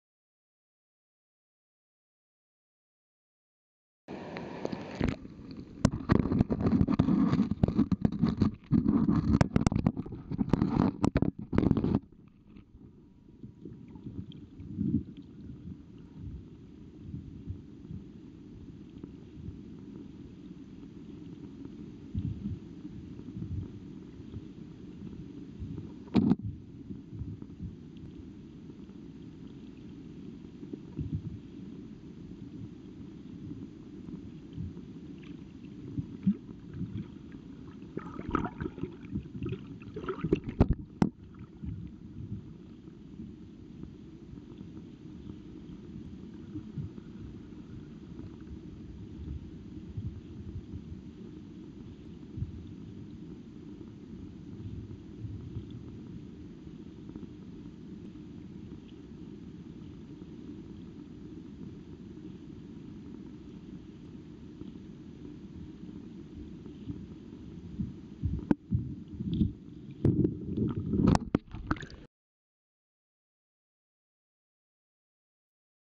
東丹沢支流 H川
雨降りしきる中、水中撮影を試みました
何とか、ヒットさせることができました。

映像が今一奇麗でないのが残念です